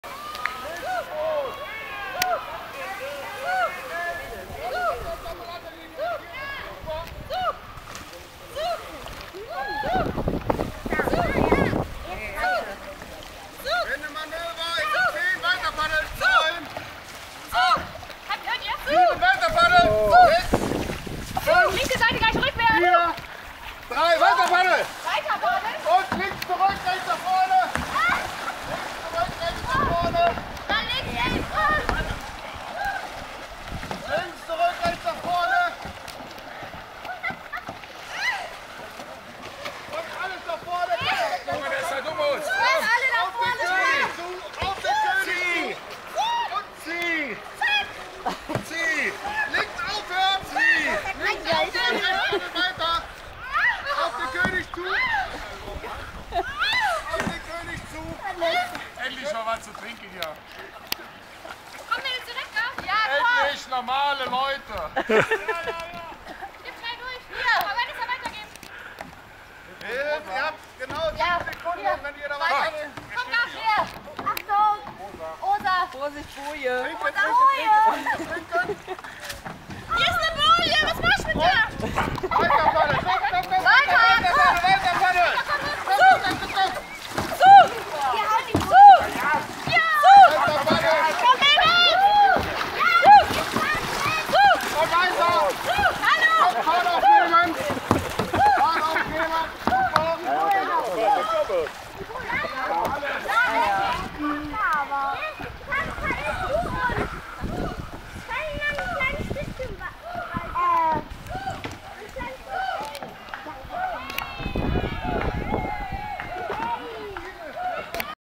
Boot 10 beim Canadierrennen 2017 in Wevelinghoven
Das 10. Boot beim Wendemanöver.
Bitte den Mannschaftsnamen in die Kommentare schreiben, wird dann nachgetragen.